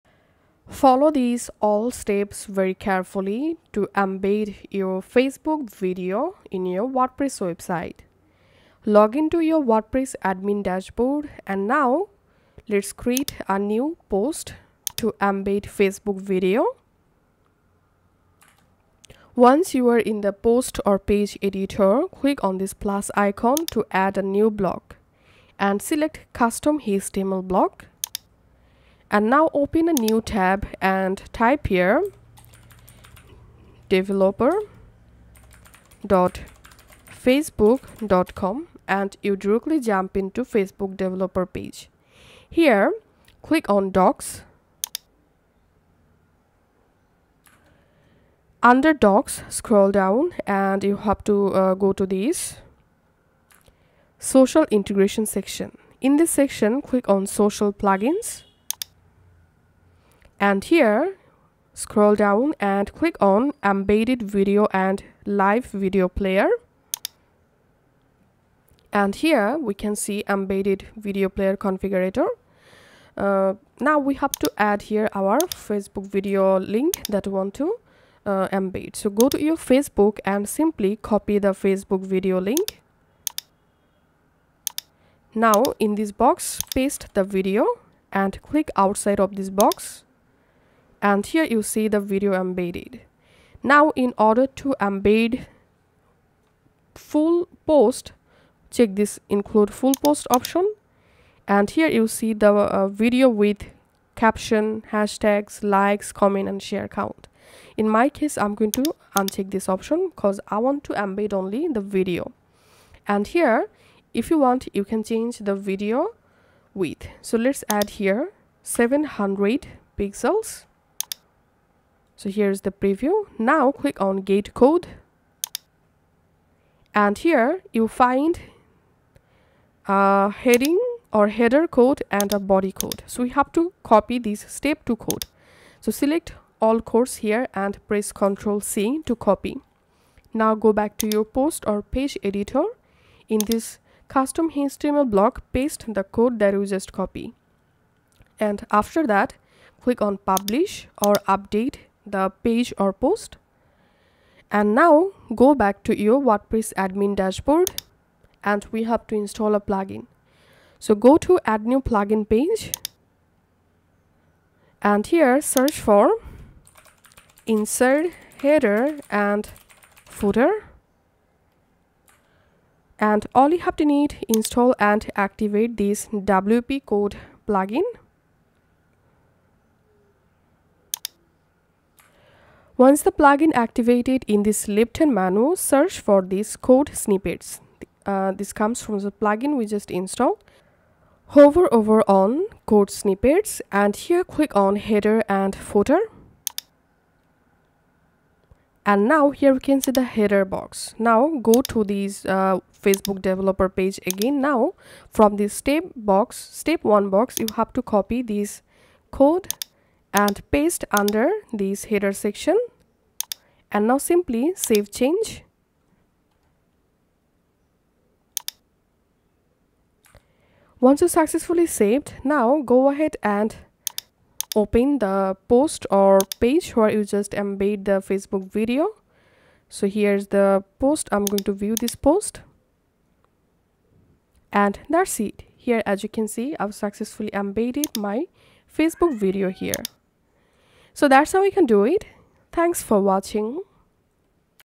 How to embed facebook video in wordpress (2025)
Learn how to easily embed Facebook Videos in WordPress posts, pages, and sidebars and get more likes and recognition for your videos.